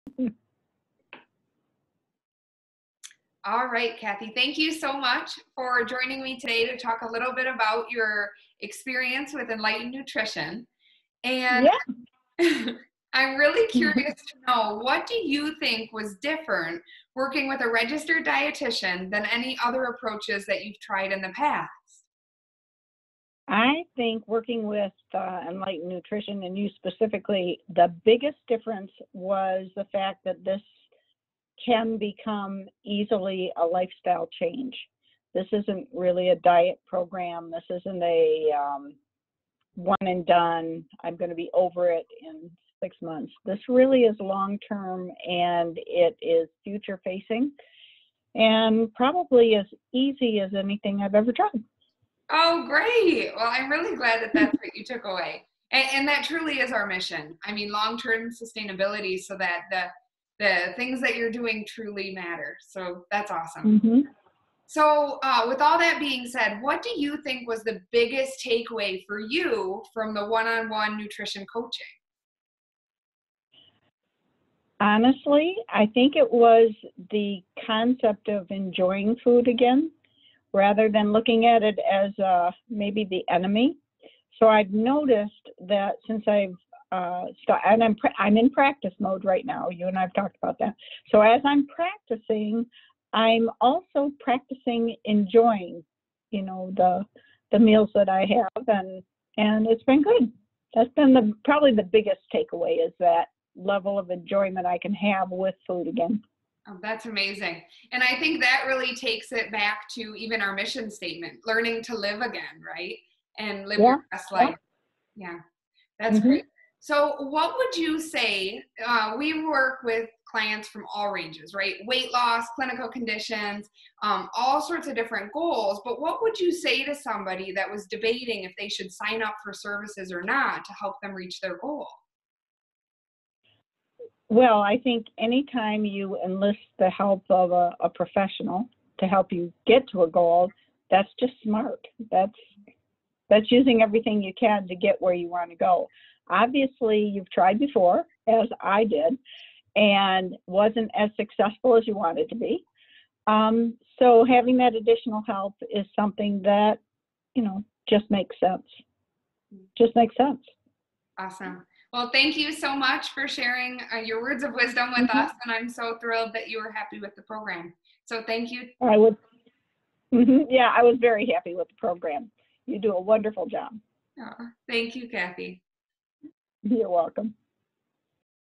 1:1 Client Success with Enlightened Nutrition
Tune in to hear more about the success this client found with Enlightened Nutrition Registered Dietitian, Taylor Schomaker!

Are you interested in signing up? Visit enlightenednutri.com and fill out your private coaching application today!